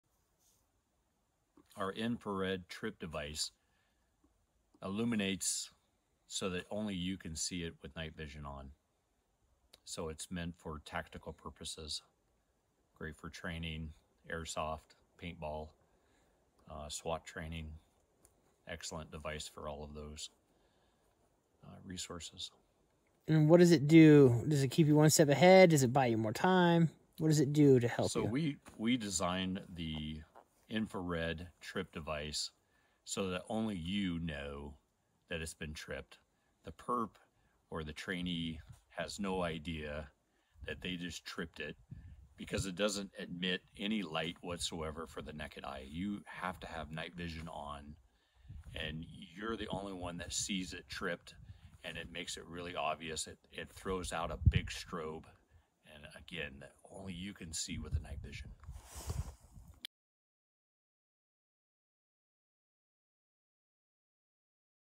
Advanced Fith Ops™ Infrared Silent Perimeter Trip Alarm
Description:
Introducing our state-of-the-art Infrared Perimeter Silent Alarm – the ultimate solution for bolstering your security! This innovative device serves as a tripline-activated early warning intrusion detector that ensures the safety of your property. It operates discreetly, alerting you to potential threats while keeping intruders oblivious to their triggered alarm signal.

Key Features:

• Invisible 940nm Infrared Technology: Our system employs cutting-edge infrared light technology, emitting a 940nm signal that's completely imperceptible to the human eye. However, it's highly visible to night vision scopes and low-light cameras.

• Unique Pulse Rate: Our alarm system generates a distinct pulse rate, making it easily distinguishable from other ambient light sources in the vicinity.

• Remarkable Visibility: With 3rd generation night vision viewers, the alarm signal can be spotted up to a mile away, ensuring you're alerted well in advance.

• User-Friendly & Durable: Easy to operate and built to withstand various weather conditions, this alarm system is designed to keep you secure in any situation.

• Reliable Power Source: Powered by a single 9-volt battery, you can trust in the long-lasting performance of this device.

• Compact and Easy to Use: Measuring at just 5" x 1.9", our alarm system is designed with space-saving in mind and proudly manufactured in the USA.

What's Included:

✔ Mounting Hardware: Everything you need for a hassle-free installation.

✔ 75 ft Spool of Dyneema® Trip Line: A durable, high-strength trip line for your security needs.

✔ Camouflaging Sleeve: Keep your alarm discreet and seamlessly blend it into your surroundings.

✔ 9-Volt Battery: A reliable power source to keep your system operational.

**Night Vision Not Included**

Enhance your security with our Infrared Perimeter Silent Alarm. Don't leave your safety to chance – invest in advanced technology that keeps you protected 24/7.